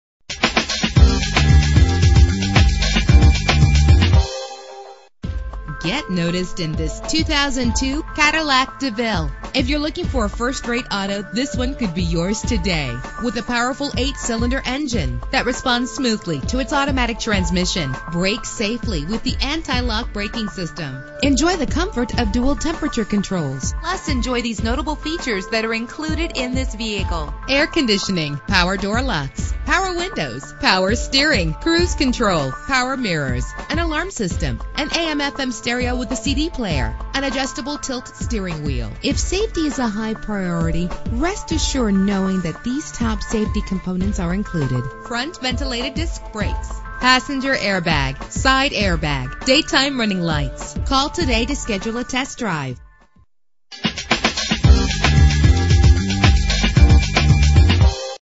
2002 Cadillac DeVille - South Jordan UT
Year: 2002
Make: Cadillac
Model: DeVille
Trim: Base
Engine: 4.6 liter 8 cylinder 32 valve
Transmission: 4-Speed Automatic
Color: Blue Onyx
Mileage: 113018
Address:
10487 S Jordan Gateway